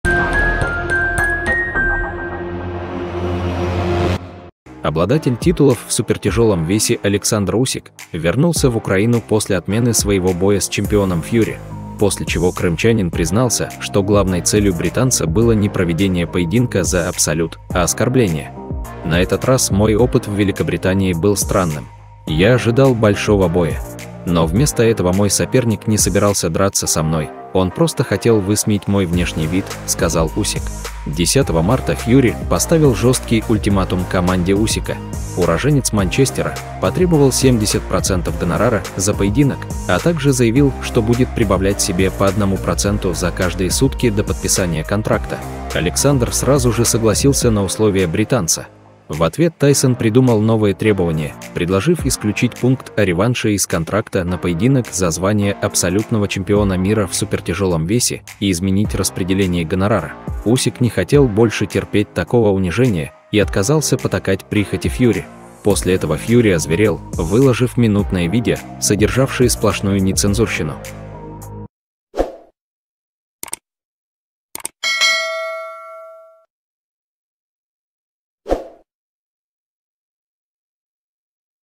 Усик Признался!! Все В Шоке!! Почему Александр Усик Отменил Бой С Тайсоном Фьюри? Никто Не Ожидал!!
Почему Александр Усик Отменил Бой С Тайсоном Фьюри
новости#новостимира#новостисегодня#новостиукраины#события#новостидня